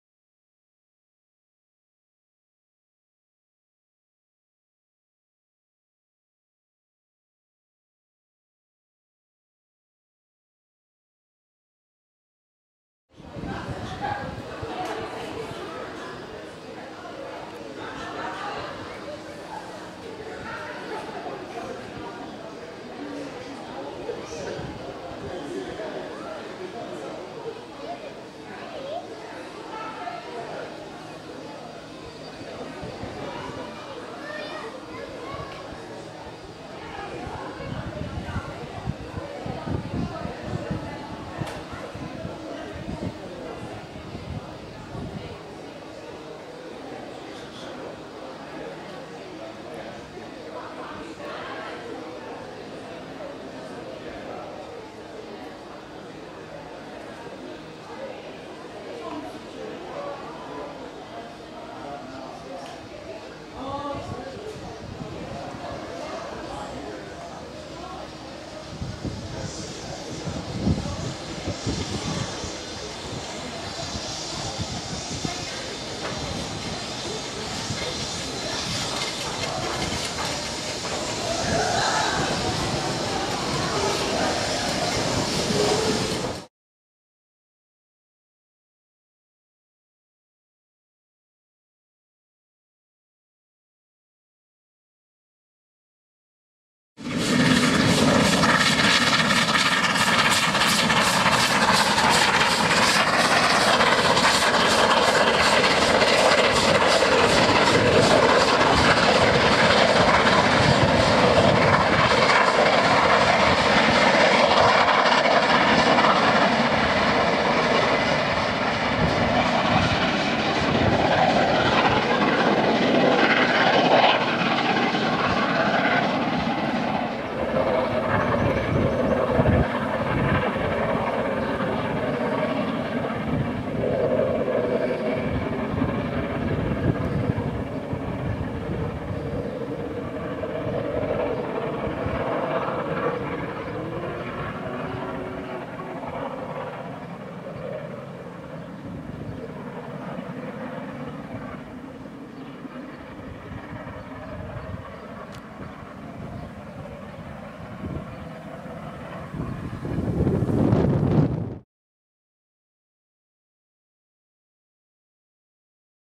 46115 Scots Guardsman - The White Rose - 26/06/2010 - HD
LMS Class 7P 4-6-0 no 46115 Scots Guardsman on The White Rose travelling from York to Kings Cross.

Footage taken at Retford Station during a water stop and also at a Signal Box just south of Retford.